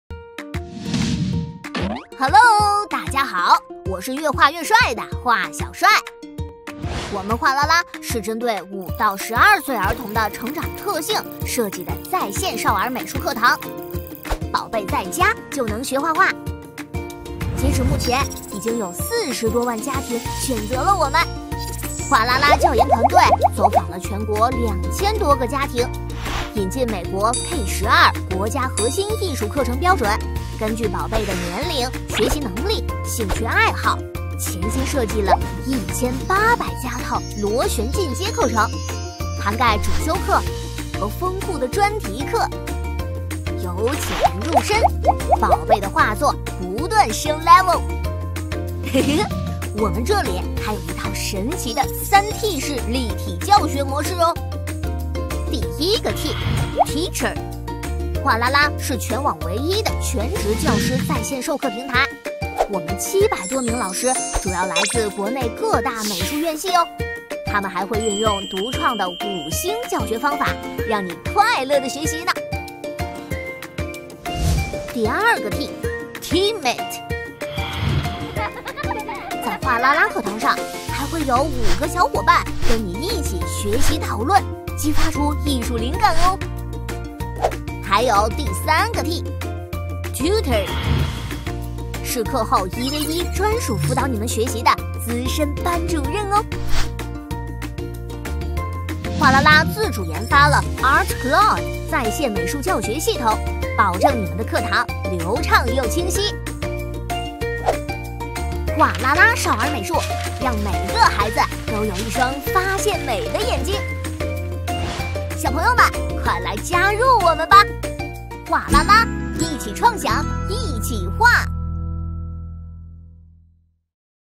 关于画啦啦少儿美术，你想知道的一切都在这里！
画啦啦，针对5-12岁的学习成长特性开发在线少儿美术课程，致力于让每个孩子都有一双发现美的眼睛。在家就能学画画！还想了解我们更多？快戳视频来看吧！